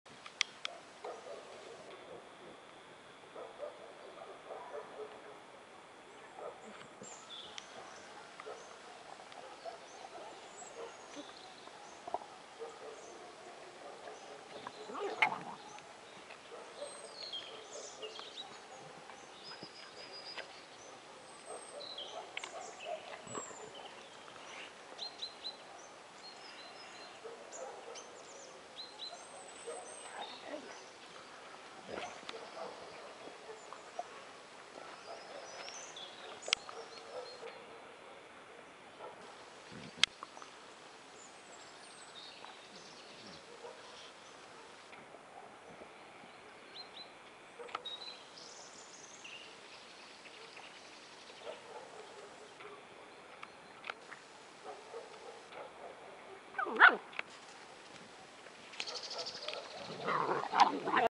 Шоколадка 06.11.17 (2)
Лабрадор, девочка 5 лет, ищет дом